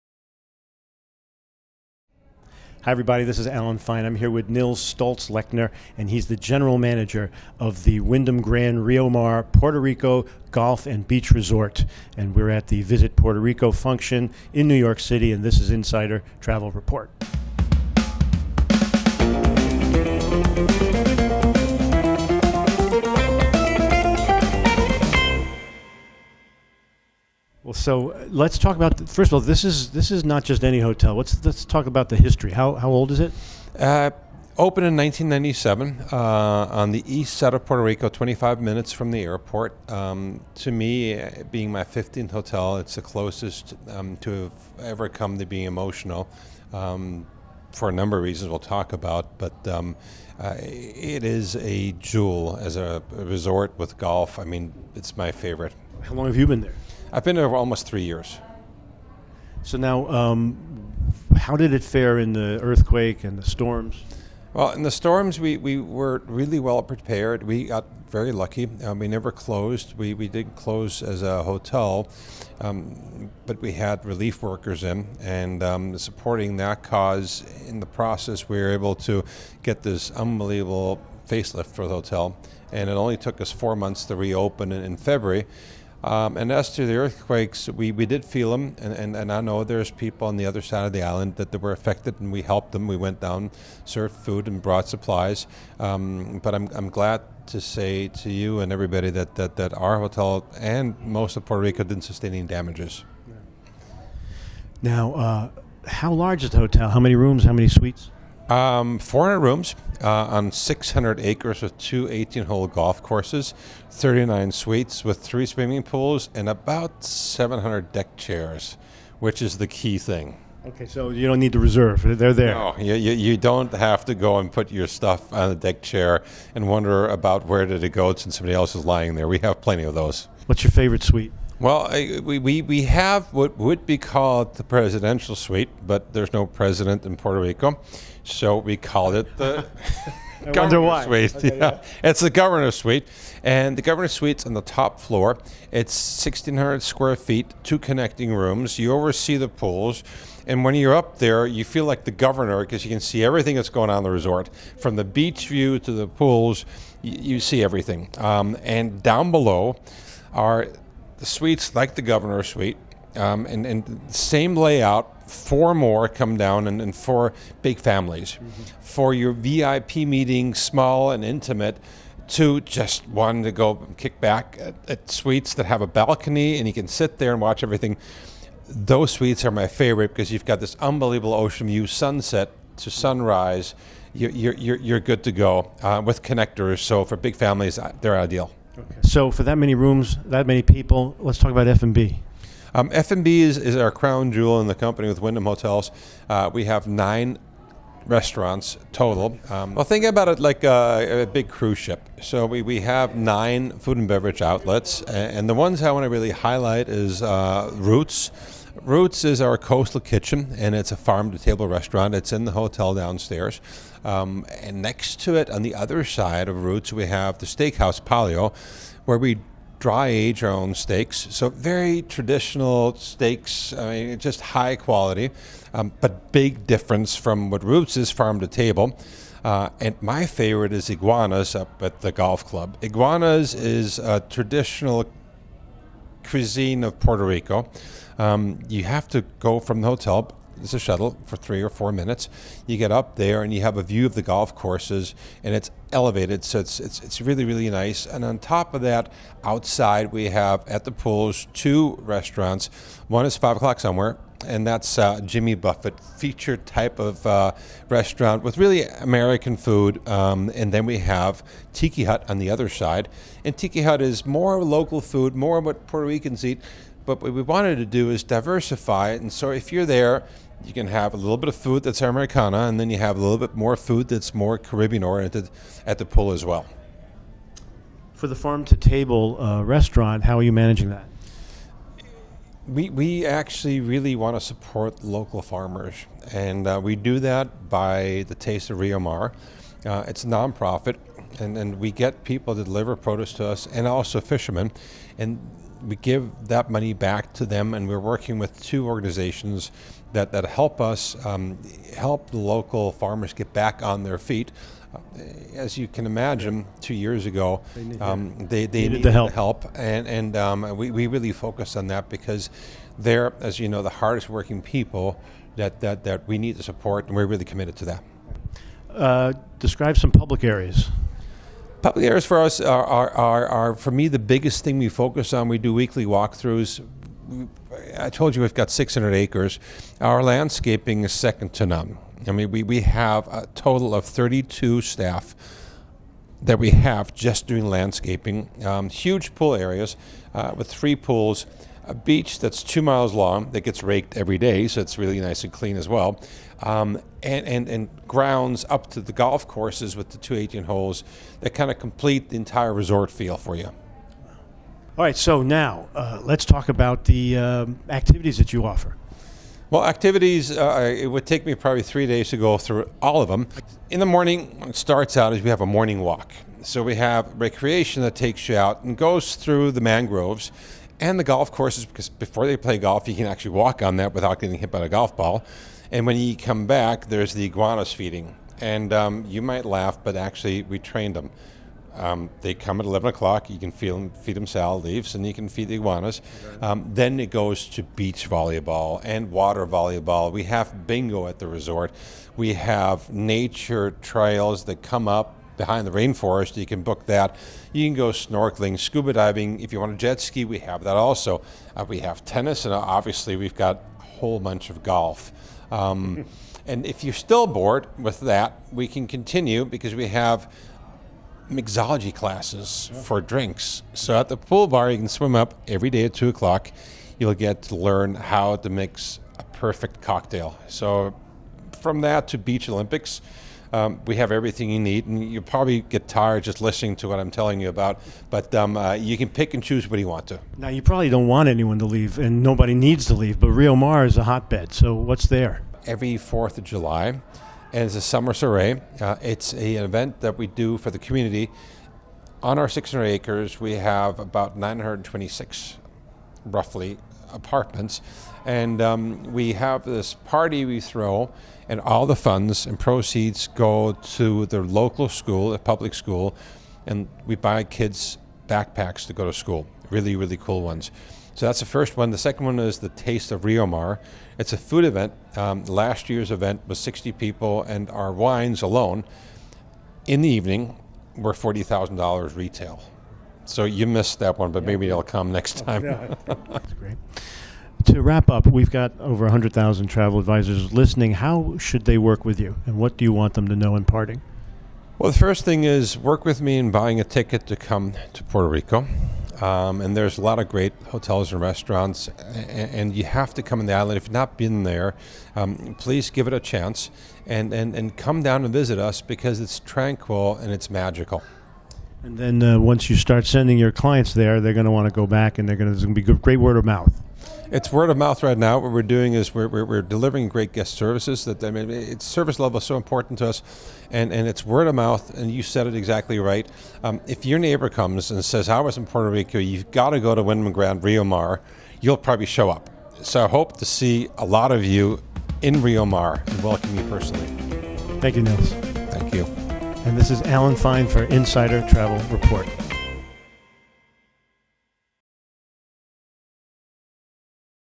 Insider Video: Wyndham Grand Rio Mar in Puerto Rico is Like a Big Cruise Ship
Nils Stolzlechner, general manager of the Wyndham Grand Rio Mar Puerto Rico Golf & Beach Resort, talks to Alan Fine of Insider Travel Report at the Visit Puerto Rico event in New York City about his hotel's 400 rooms, 39 suites and nine restaurants on 600 acres, with two 18-hole golf courses, a two mile long beach and three pools with about 700 deck chairs.